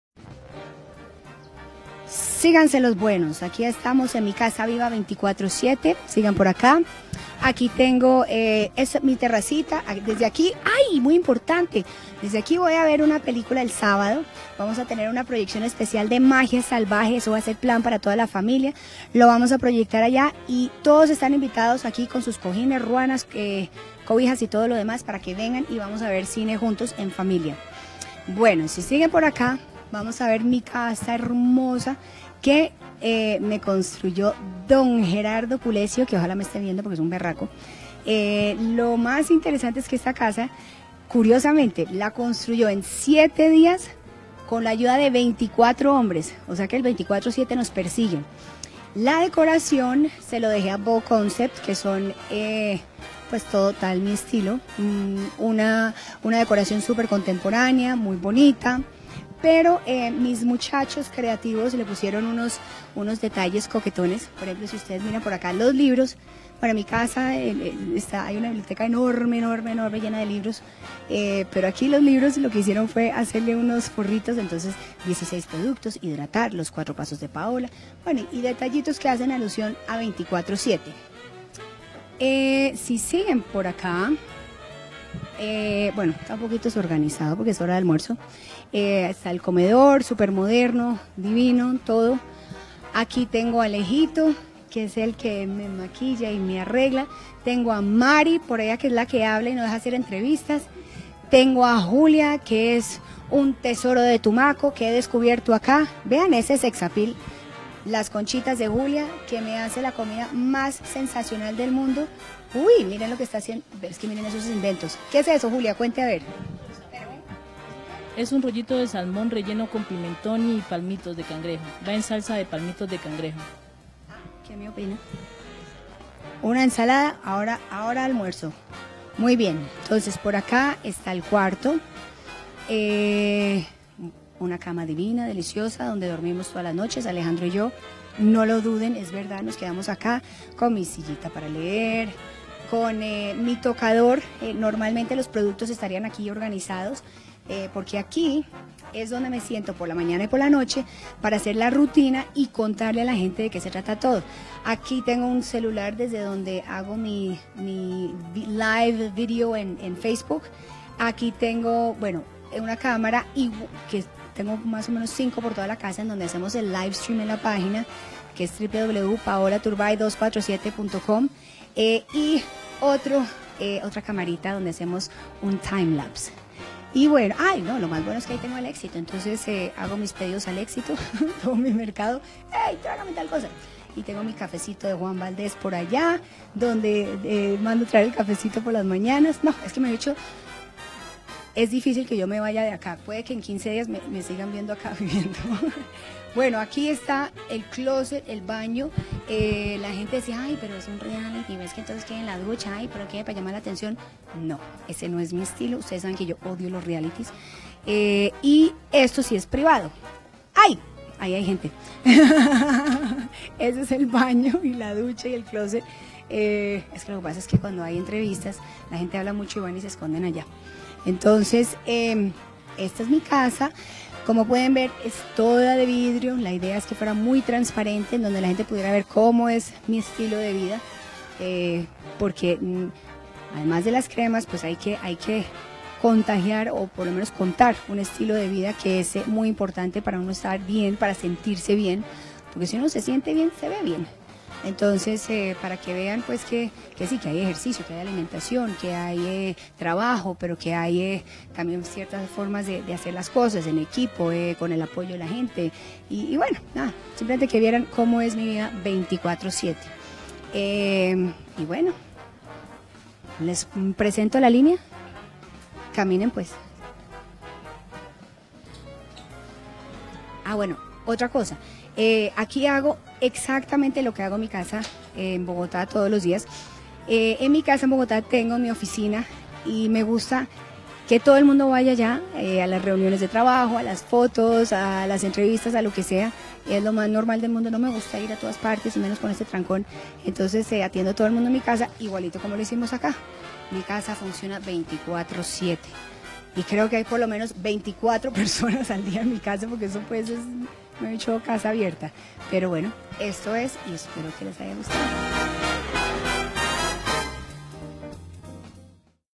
Conoce la Casa de Cristal de Paola Turbay en este recorrido
Como parte de su estrategia para lanzar al mercado su línea de belleza 24/7 by Paola Turbay, la empresaria nos mostró los secretos de su nueva casa.